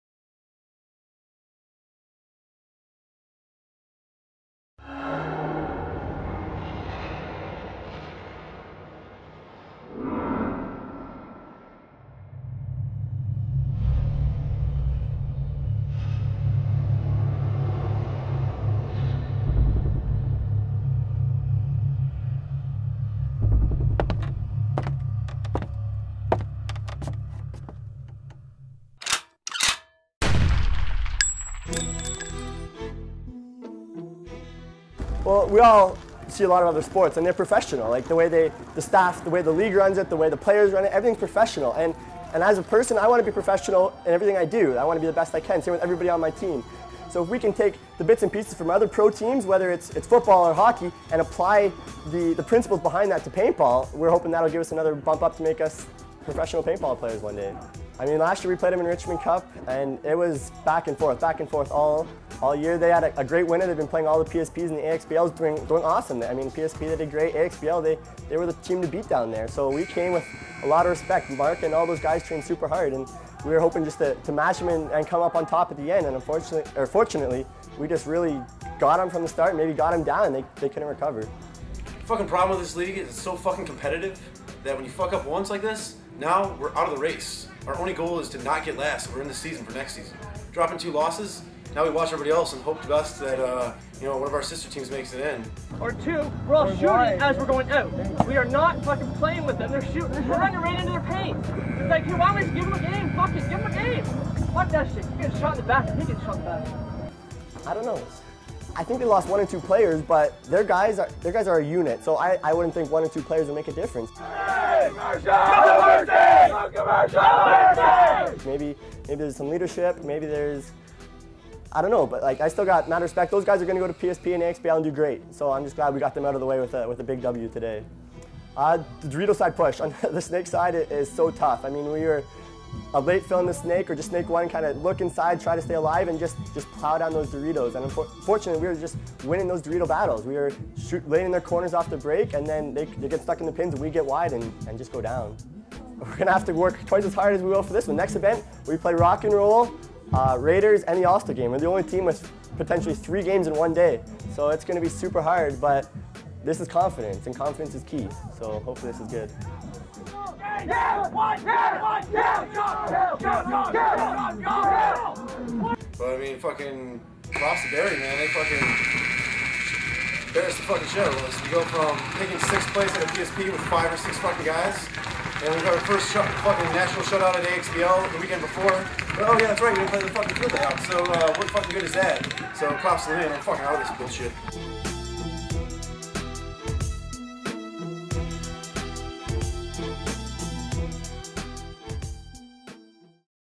Rewind CXBL 2008: E2 Central - Kingston vs Barrie
Kingston versus Barrie at CXBL Event 2 2008.  Enjoy!